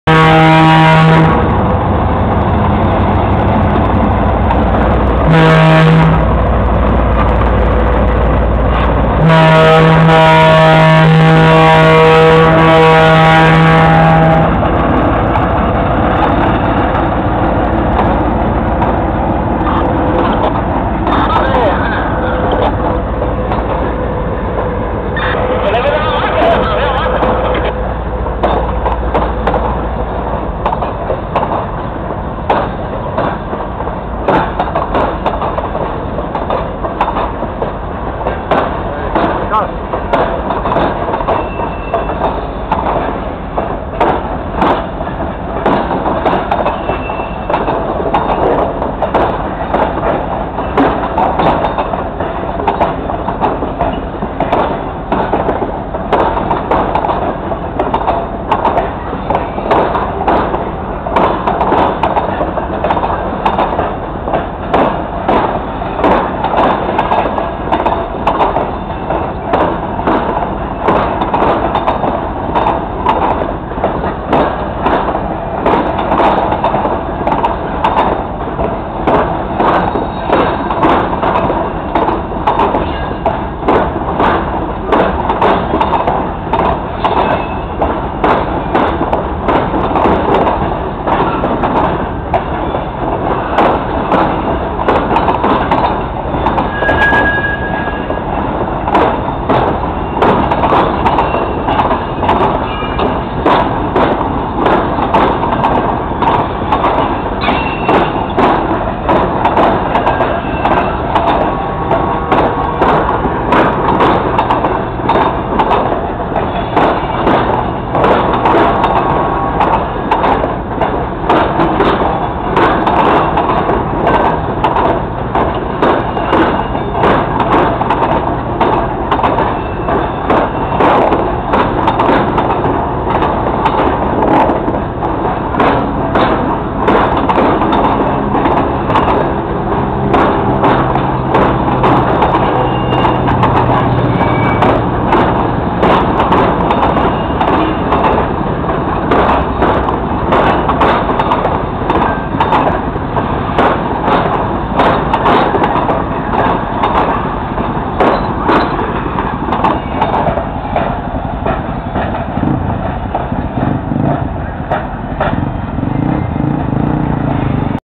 Macosa G22U rumo ao porto de paranaguá
Travessia da macosa G22U #4343 da ALL com 30 vagões graneleiros F e H.